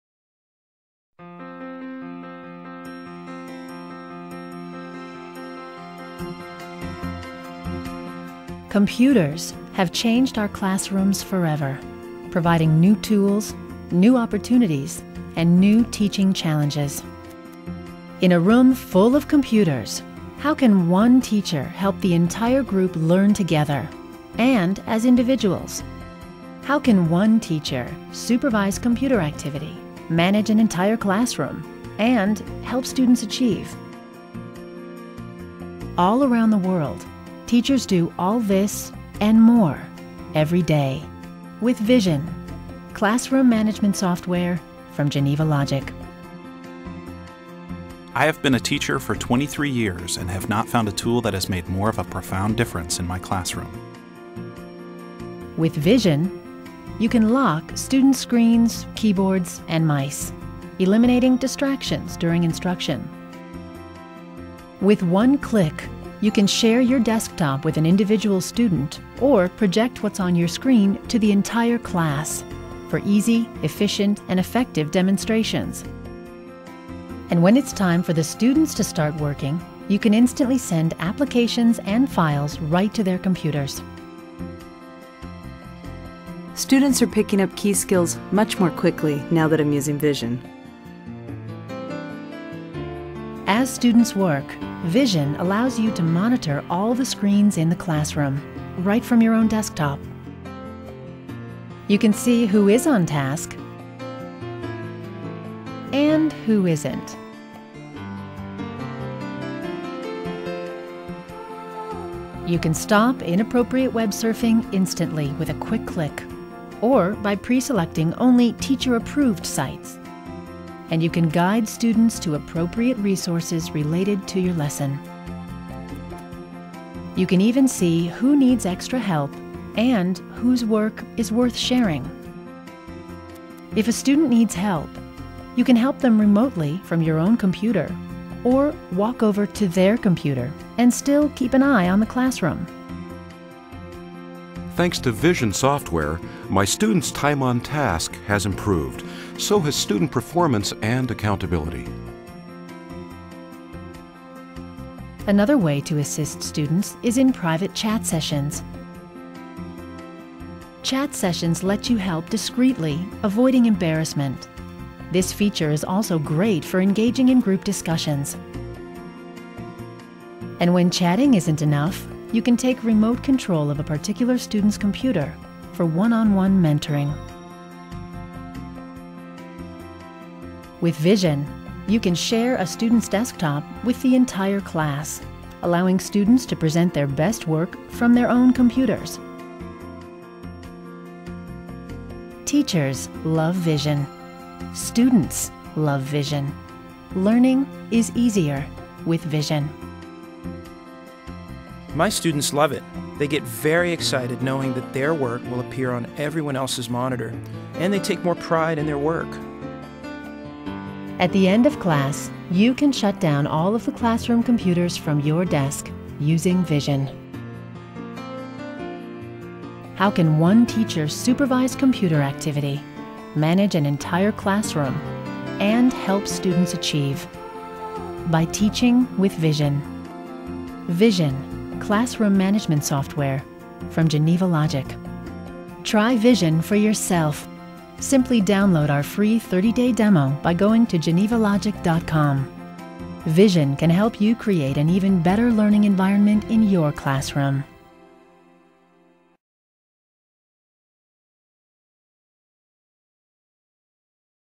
Vision Movie
See how Vision classroom management software works in a real classroom